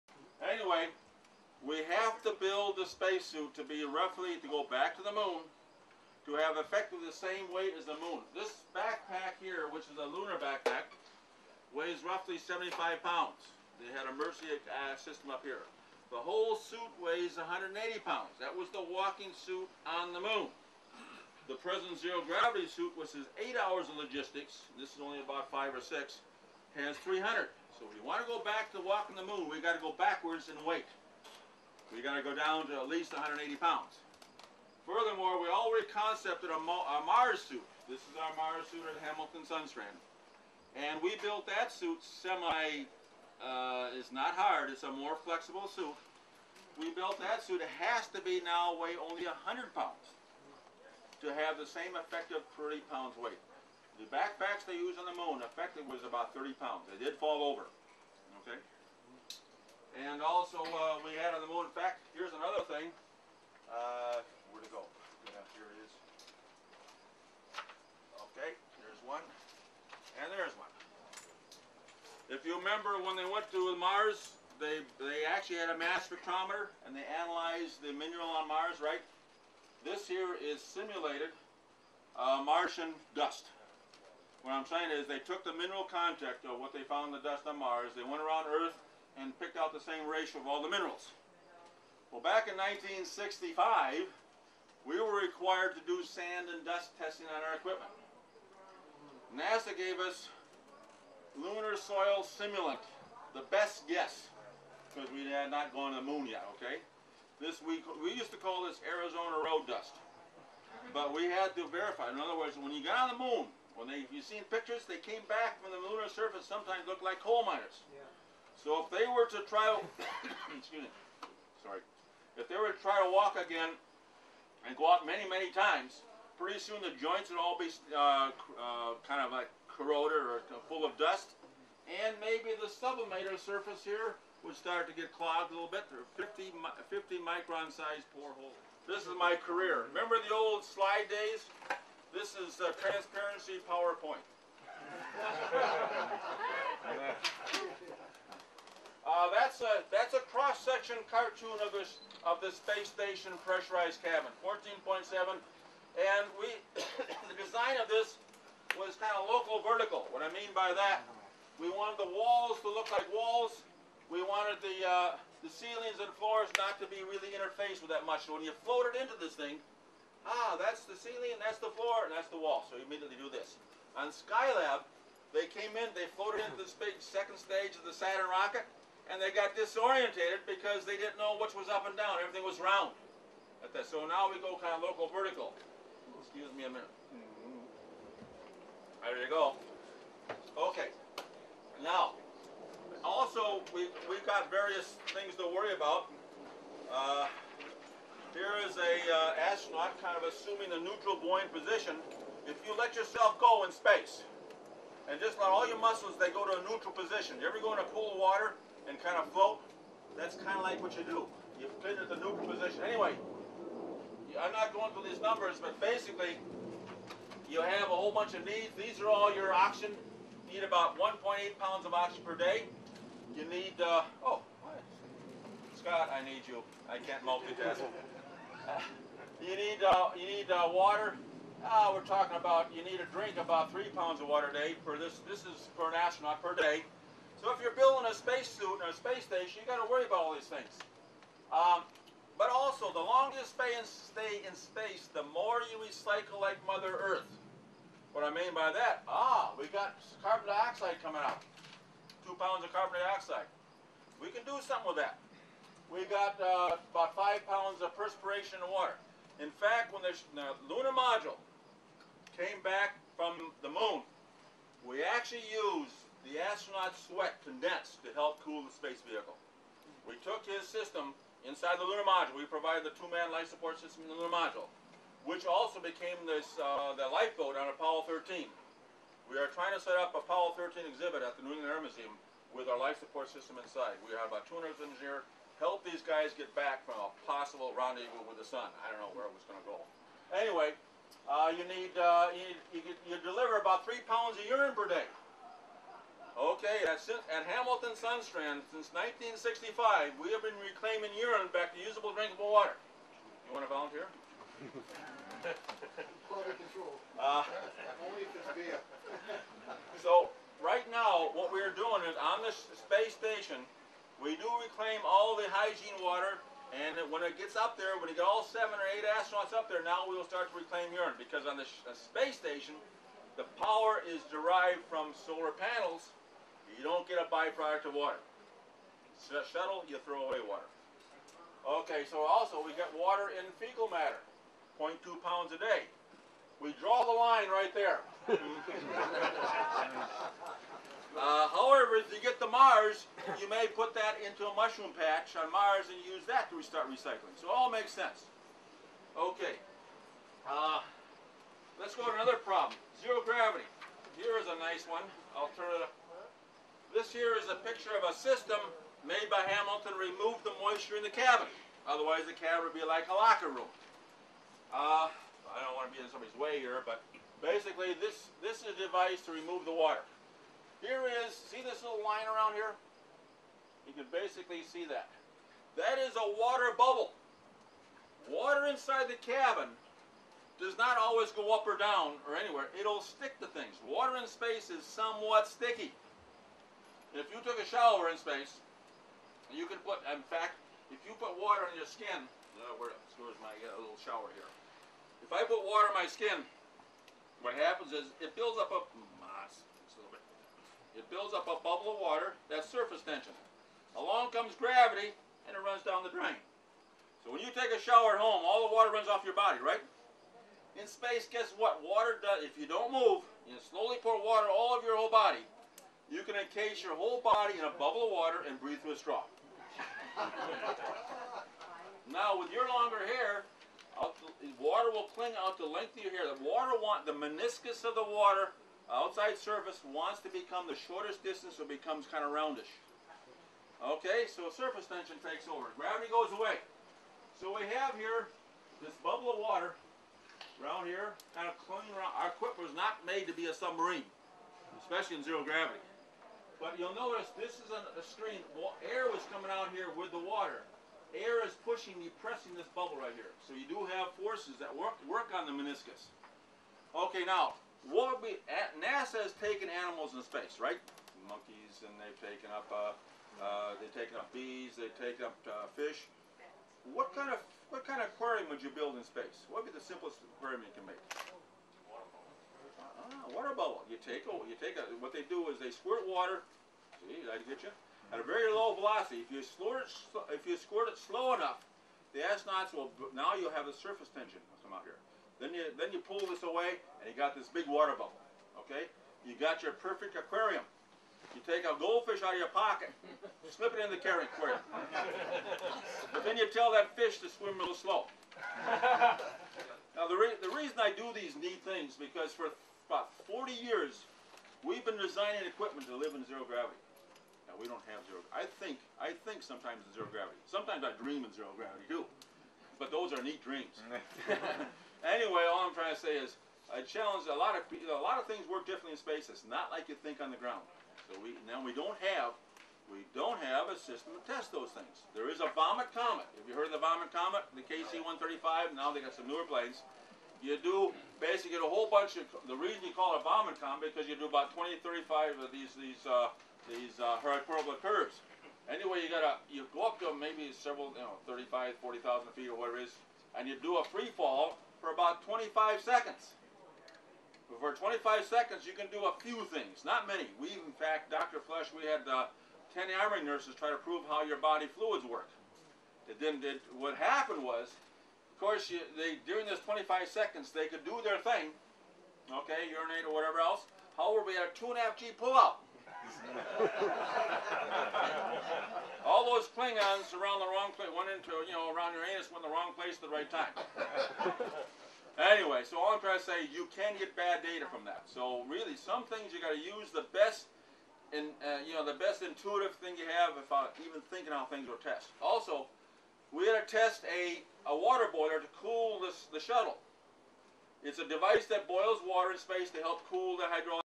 Dr Flush 05
Space suit toilet technology
Donald Rethke,  NASA engineer, aka Dr Flush